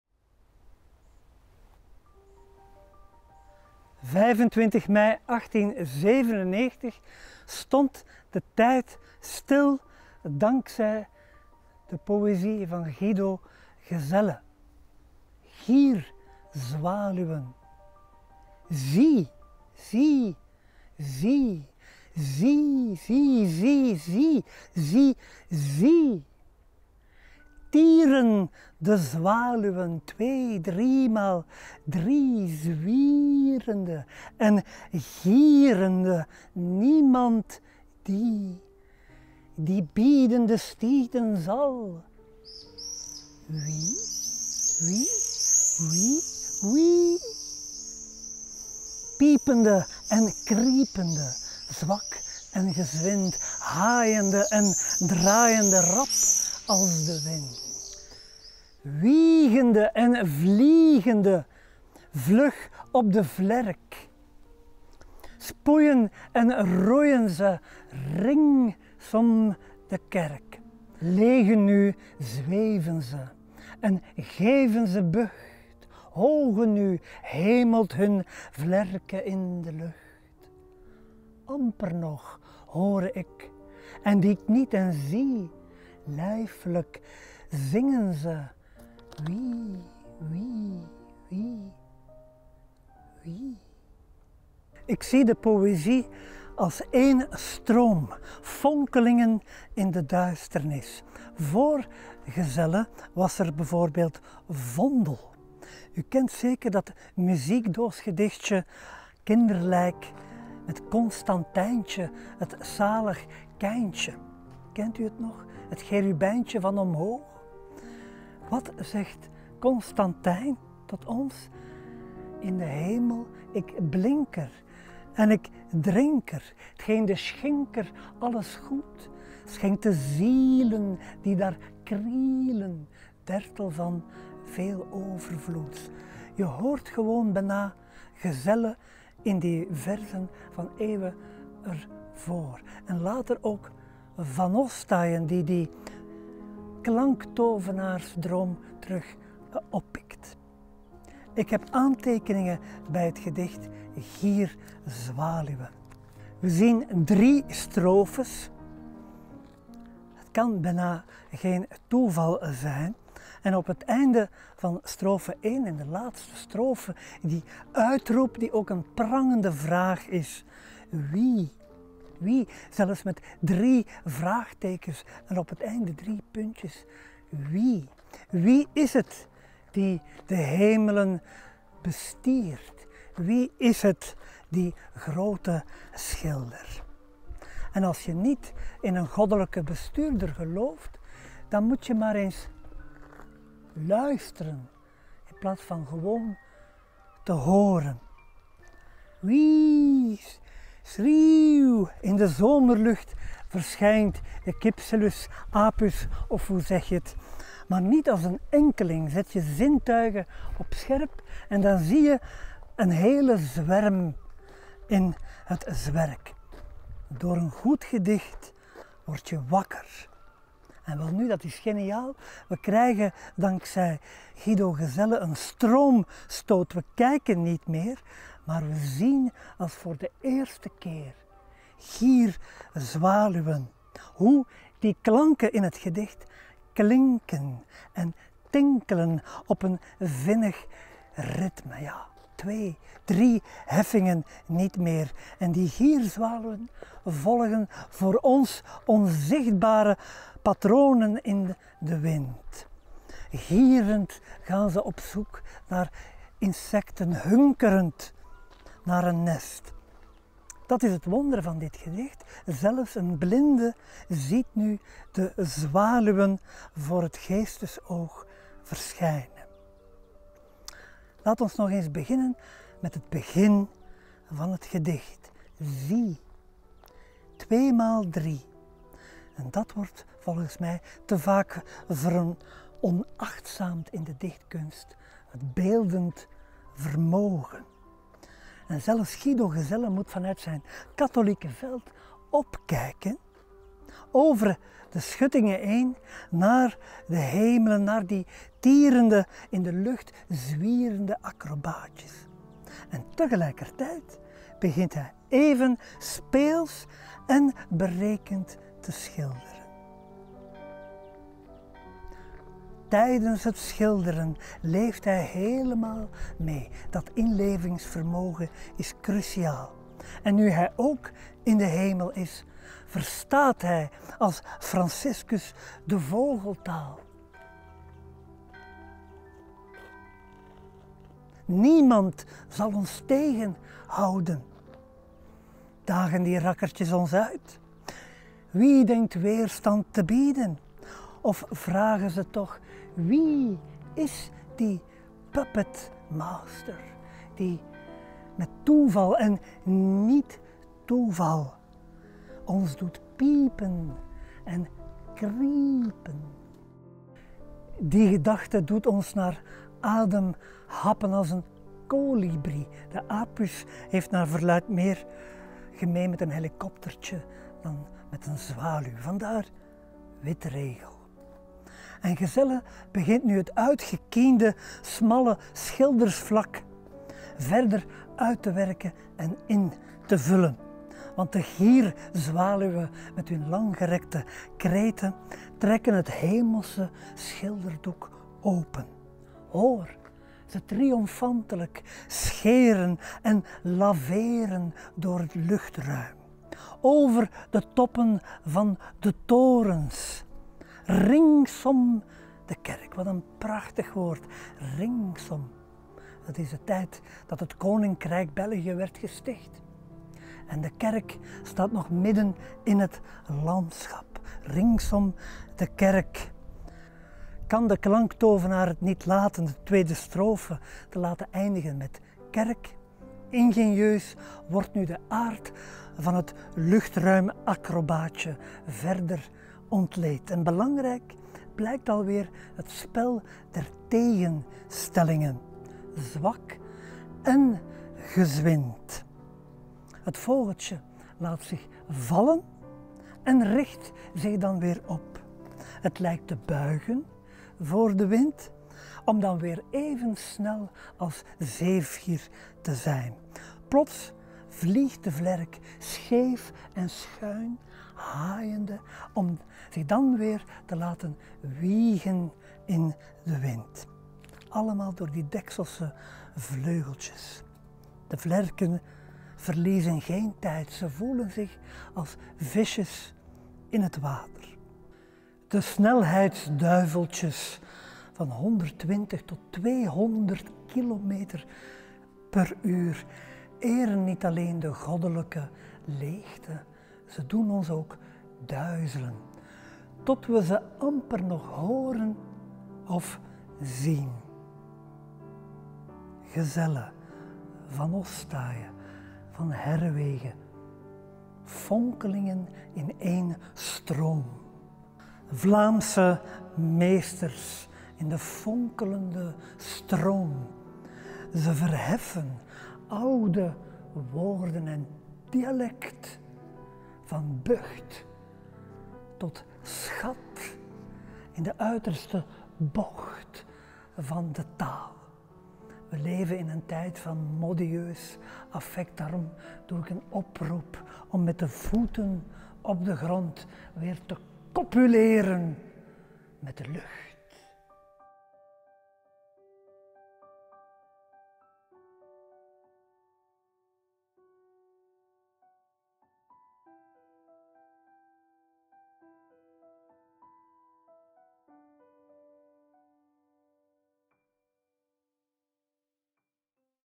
Mijmeringen bij Gezelles natuurgedichten - Peter Holvoet Hanssen
We confronteerden hedendaagse dichters met een natuurgedicht van Gezelle. Via zijn versregels connecteren ze met het heden. Wat maakt Gezelle nog actueel en zo bijzonder vergeleken met tientallen andere vergeten natuurdichters? Ze mijmeren, dichten, reflecteren en gaan met elkaar in gesprek.